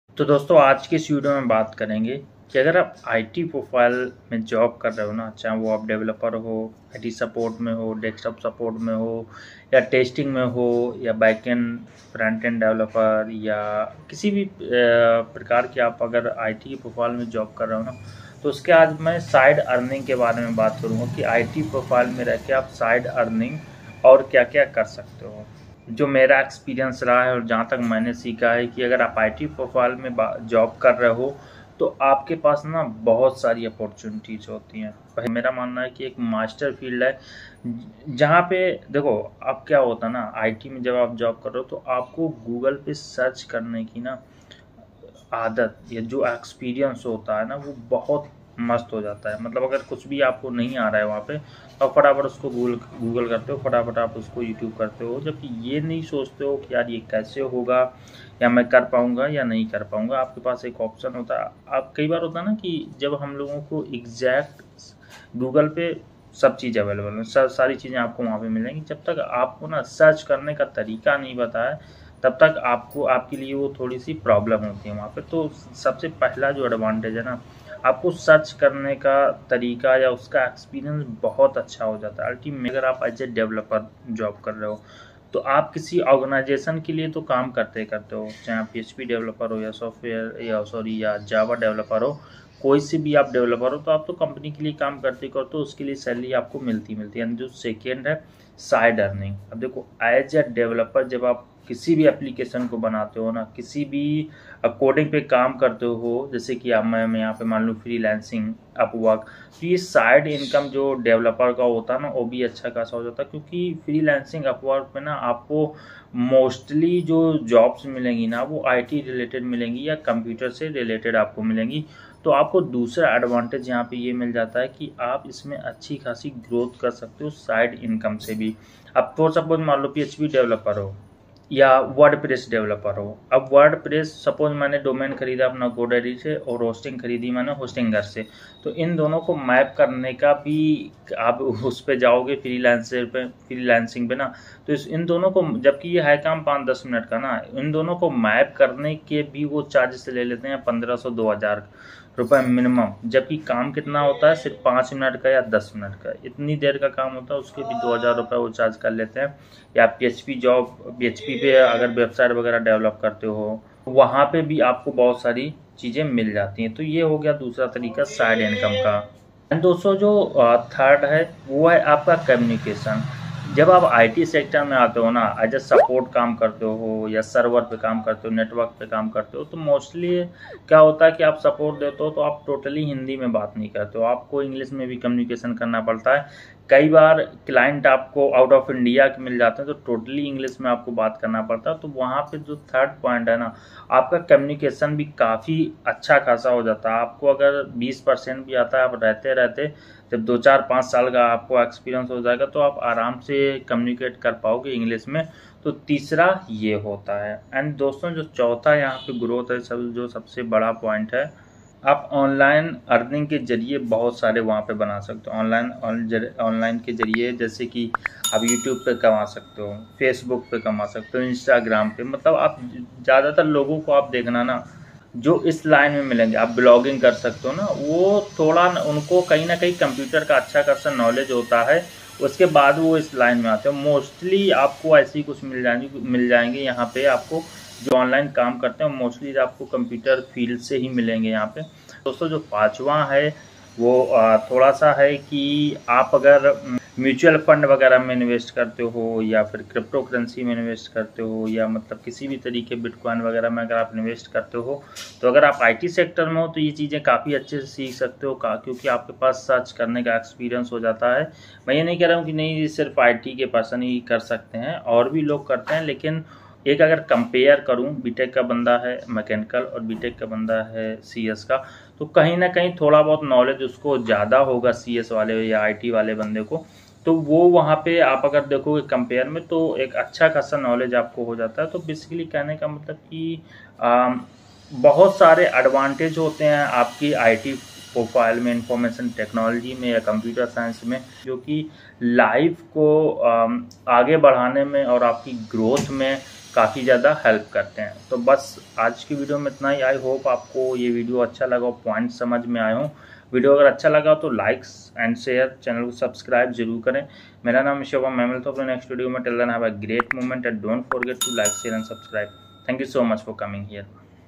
Now Earn Online 2 Lakhs/Month if you are in IT Profile or Software Developer ?Computer Jobs earning
Maine is video me btaya h ki agr aap information technology me job krte h to uske side earning kya h or kitne tariko se side earning kr sakte h agr aap IT profile ki job me h to or kitna paisa kma sakte h full detail me discuss kiya h i hope aapko video acha lage thanks Now Earn Online 2 Lakhs/Month if you are in IT Profile or Software Developer  ?Computer Jobs earning

Thanks.